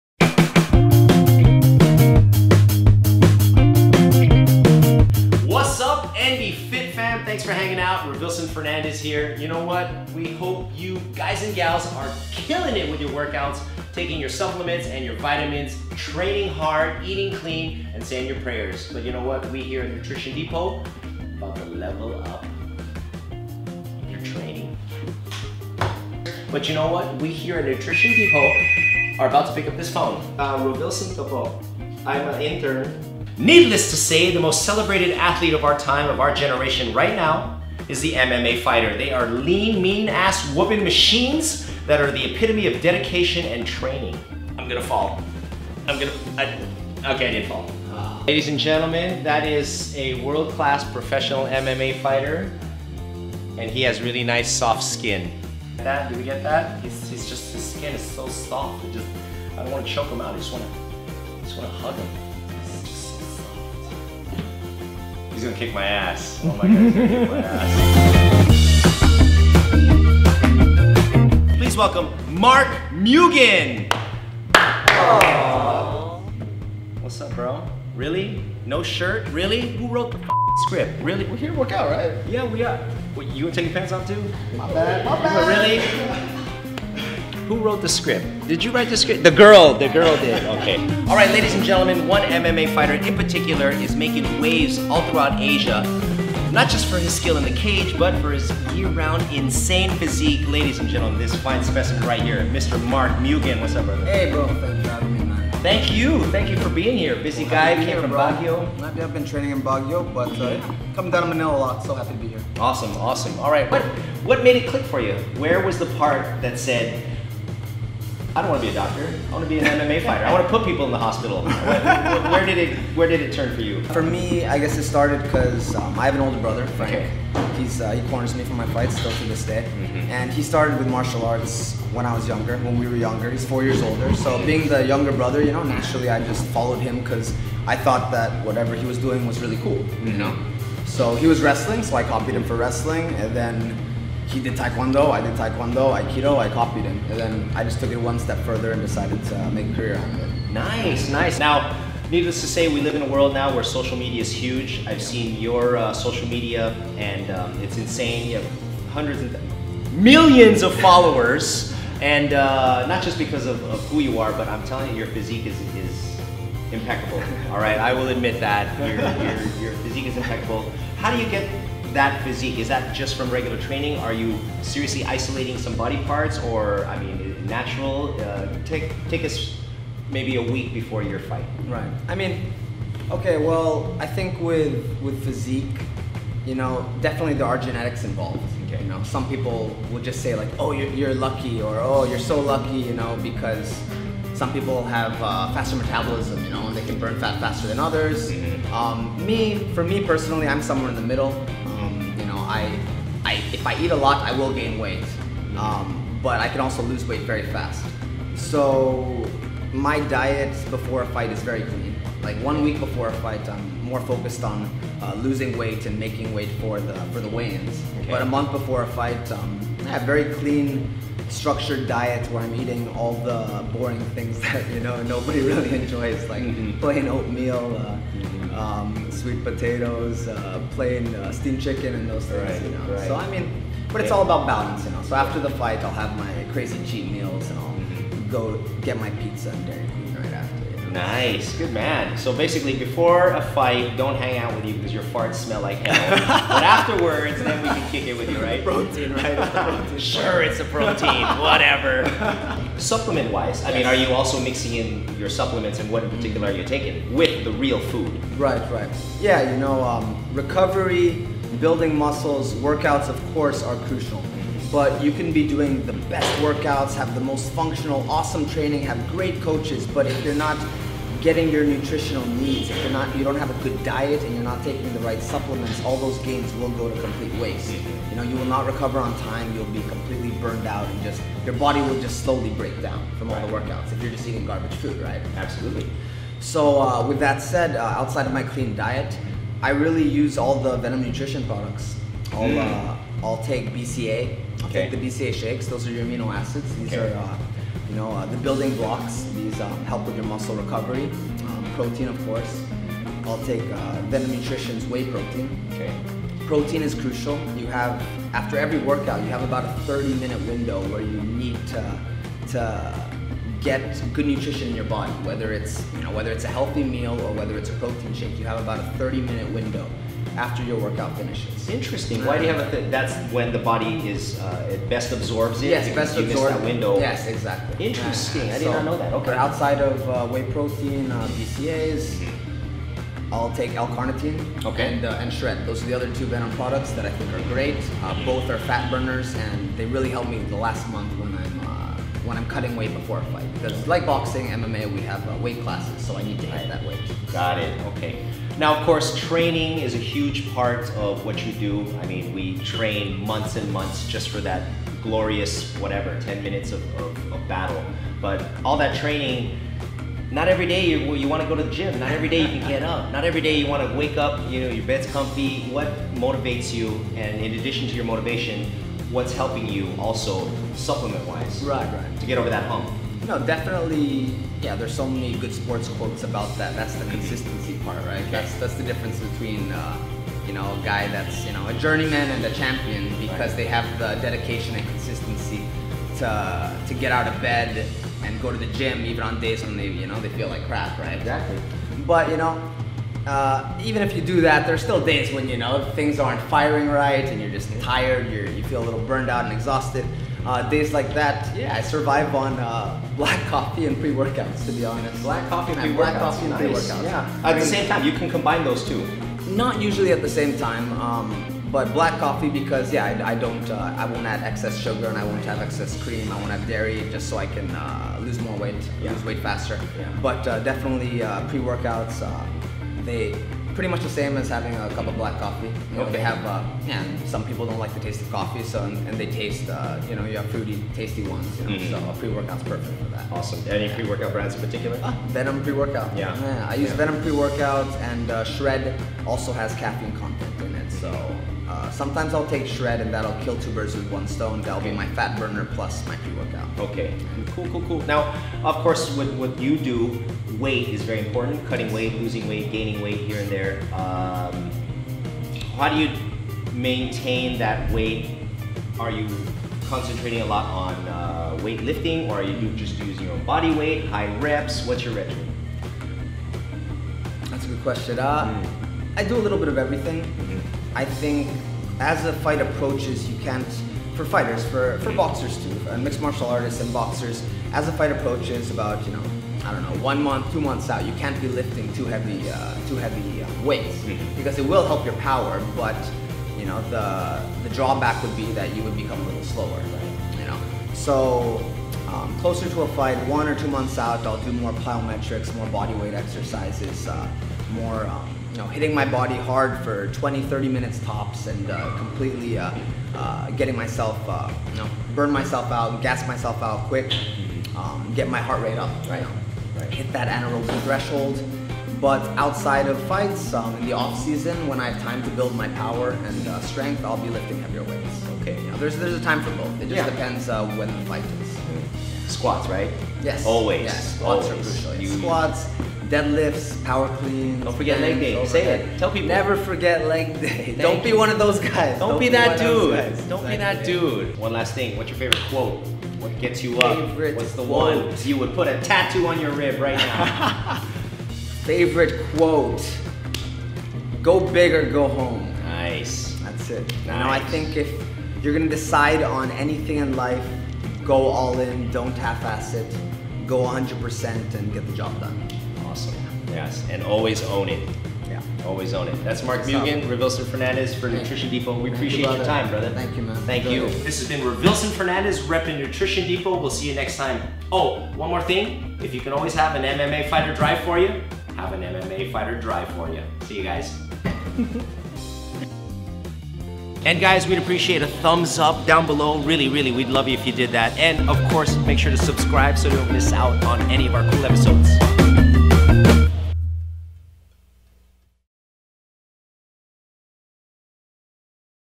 Rovilson Fernandez - Getting Lean and Ripped with Mark Mugen - Ep. 4
Get insights on how Mark "Mugen" Striegl prepares for his upcoming fight on URCC